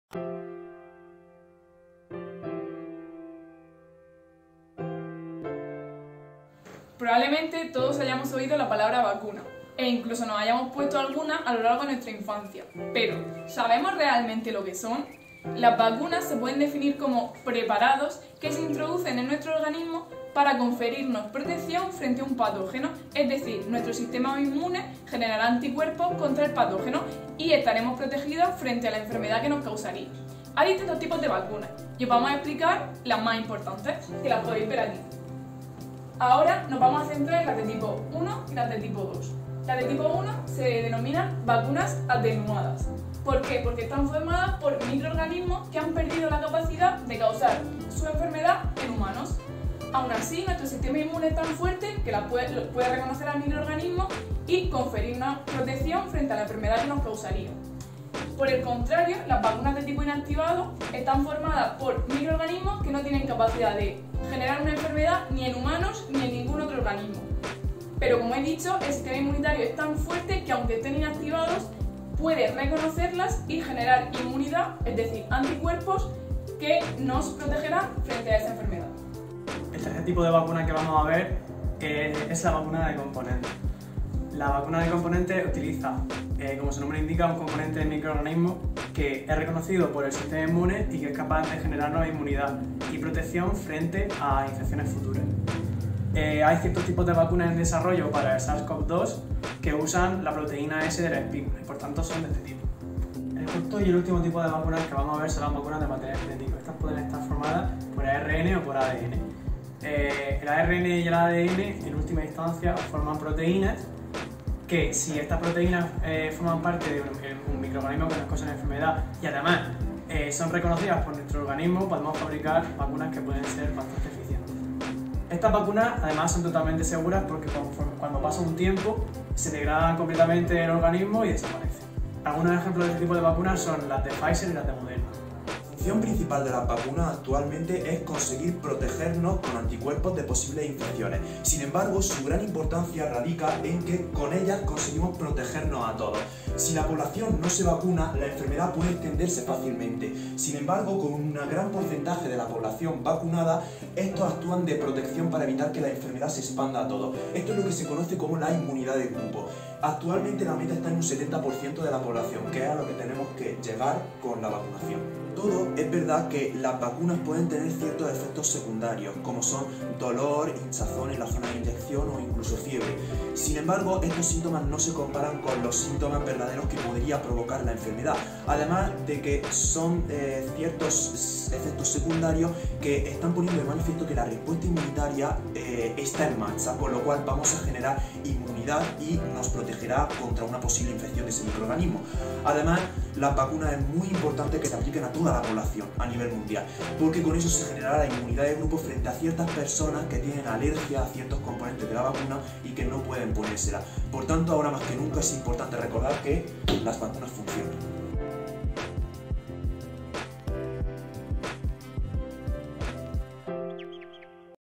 Coronavídeus19 #4: Vacunas: por qué son importantes y por qué hay que ponérselas | BiotecMur
Los miembros de BiotecMur, la Asociación de Biotecnólogos de la Región de Murcia, han realizado una serie de vídeos explicando conceptos básicos que hemos ido escuchando a lo largo de la pandemia de forma que sean comprensibles para el público general.
Hoy os hablamos de las vacunas, de por qué son tan importantes y por qué van a ser la herramienta que nos saque de esta situación.